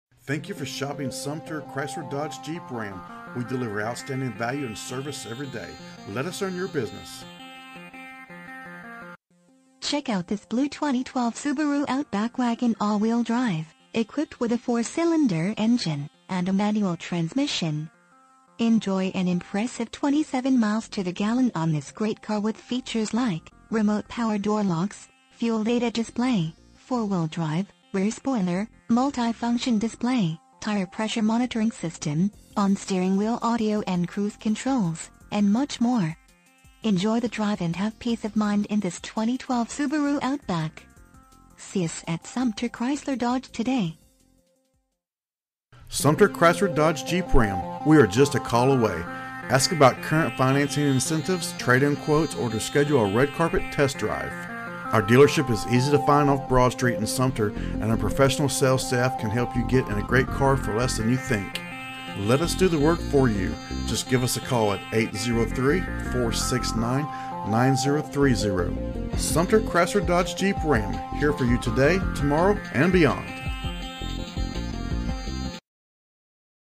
2012 Subaru Outback Sumter Chrysler Dodge Sumter, SC 29150
Subaru Outback Check out this Blue Subaru Outback 4 Dr Wagon AWD, equipped with a 4 Cylinder engine and a manual transmission. Enjoy an impressive 27 miles to the gallon on this great car with features like Remote Power Door Locks, Fuel Data Display, 4 Wheel Drive, Rear Spoiler, Multifunction Display, Tire Pressure Monitoring System, on steering wheel audio and cruise controls, and much more. Enjoy the drive and have peace of mind in this Subaru Outback. See us at Sumter Chrysler Dodge today!
MILES: 33,270
VIN:4S4BRCAC2C1287862

2662 Broad Street
Sumter, South Carolina 29150